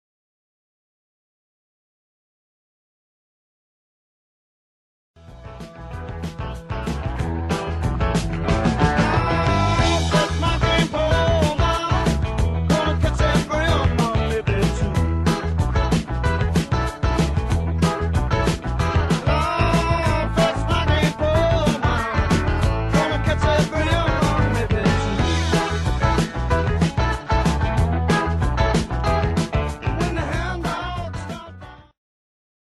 bream2
Bosshawg and JC catch some nice bream in Buhl, AL on Micro-Lite spinning reels by Okuma and tiny crankbaits.